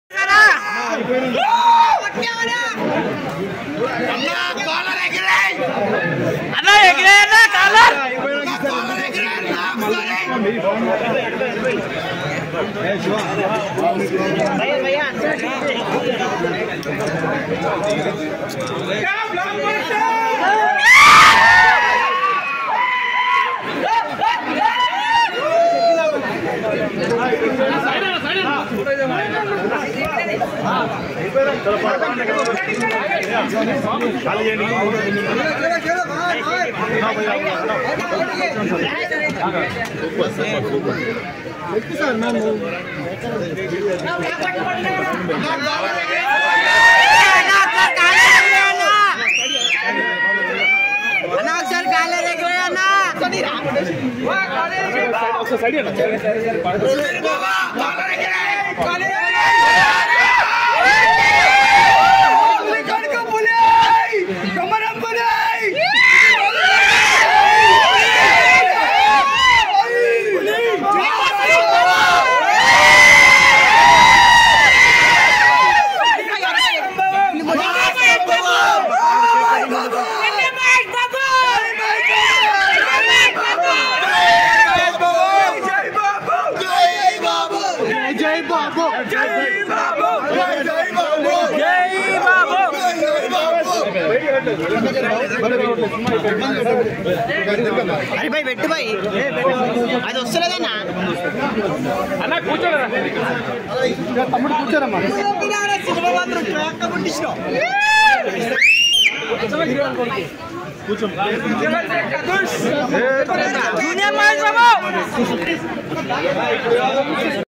Harom Hara Movie Sudheer Babu Success Celebrations at Sandhya 35MM | Harom Hara Public Talk| Jr.MB
Harom Hara Movie Success Celebrations At Sandhya 35MM Sudheer Babu and Son Jr. super Star